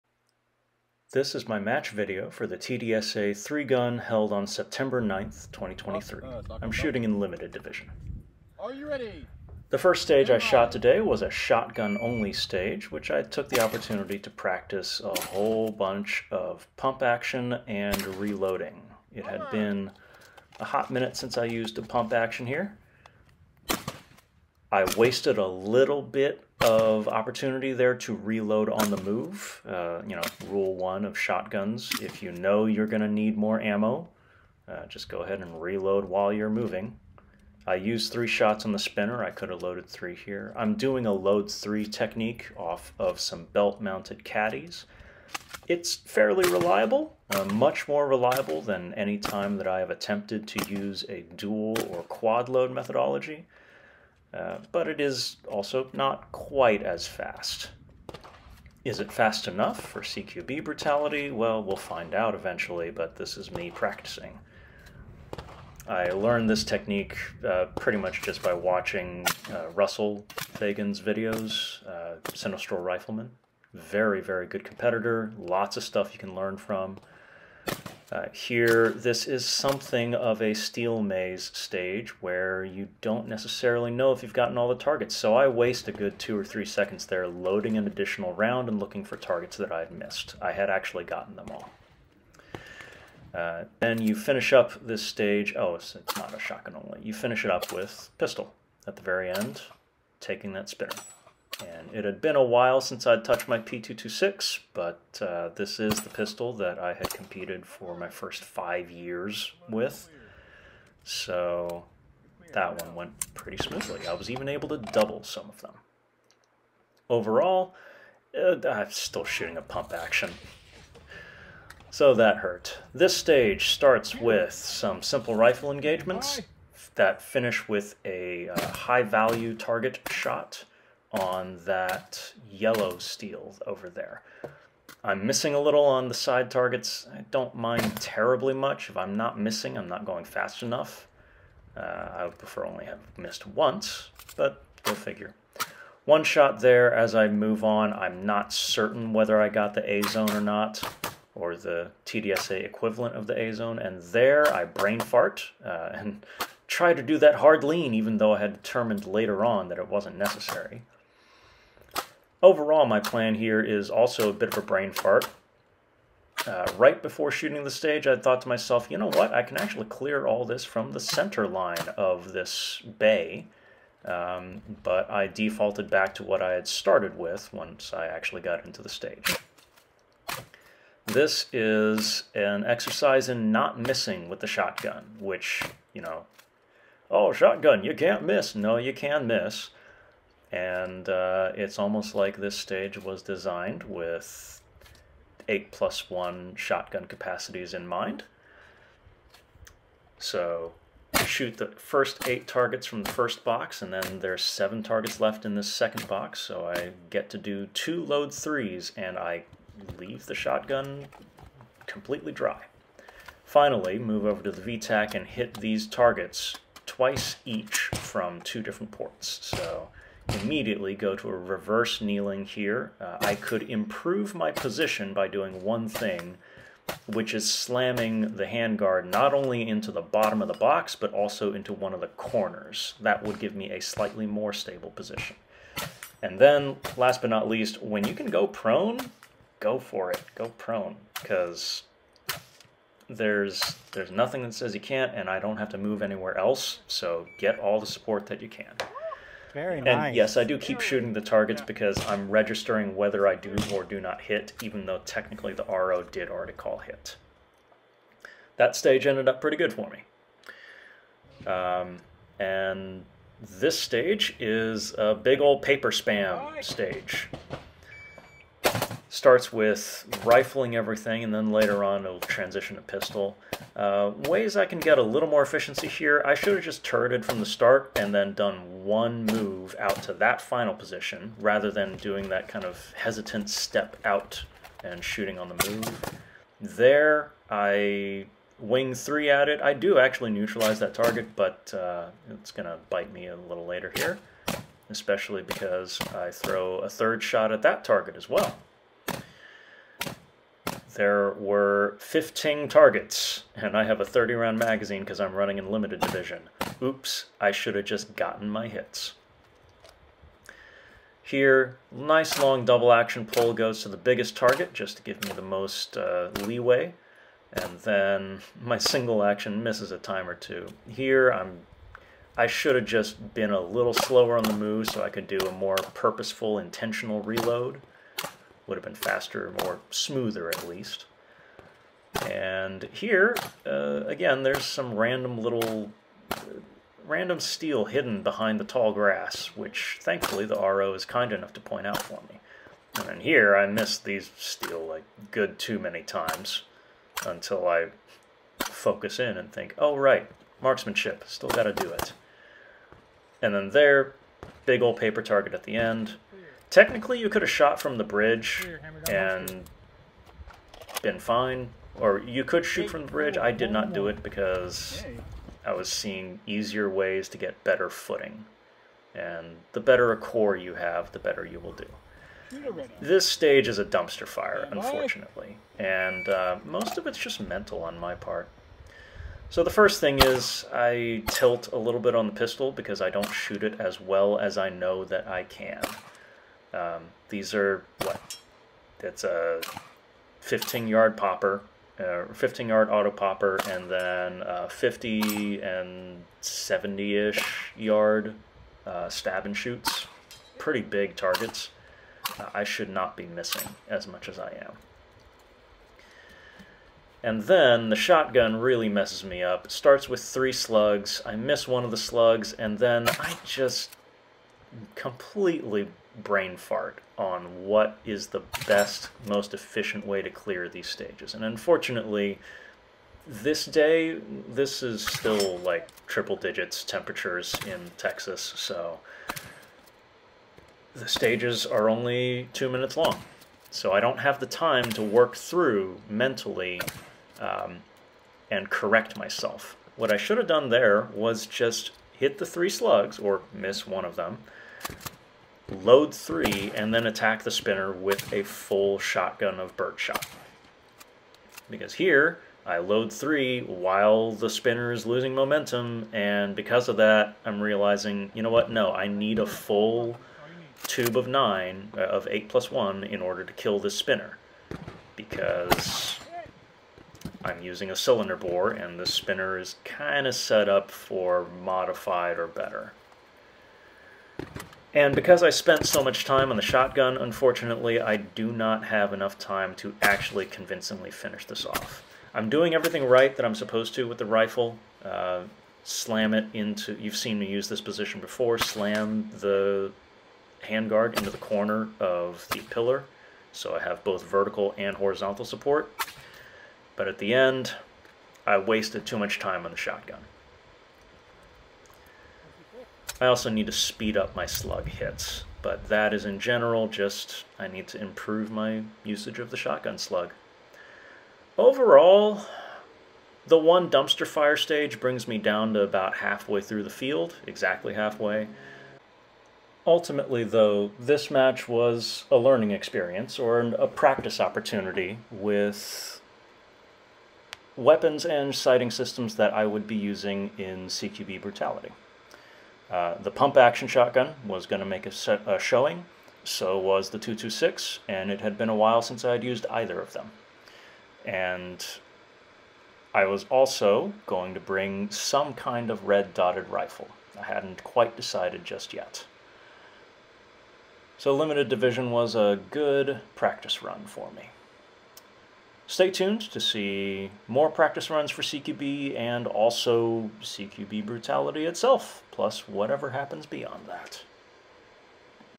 TDSA 3 Gun, Sept. 9, 2023 - All Stages
Range: Texas Defensive Shooting Academy (TDSA) in Ferris, TX
Outfit: Varusteleka Combat Shirt & Pants in Finnish M05 camouflage
Rifle: Home-assembled 14.5" WWSD rifle w/ Holosun HS503-ACSS
Pistol: Sig Sauer P226 "Enhanced Elite"
Shotgun: Mossberg 590 w/ ghost ring sights

September in Texas means it's still triple digits, there are only 4 stages, and the par times are a tight 2 minutes so that we all might clear out of the range by Noon-ish.

This match was a chance to practice very specifically weapons systems and sighting systems I intended to use at CQB Brutality - the last time my Mossberg had come out, I had not performed that hot with it and I needed to get more reps. Same went for my P226, the heart and soul of my competitive pistol shooting through 2020, but which I hadn't touched meaningfully since 2020.

Overall, I got in very good reps on my guns and equipment, and I was humbled by one dumpster-fire of a stage reminding me that I need to treat these harder-to-use guns with their due respect and intelligence in order to clear these stages on time.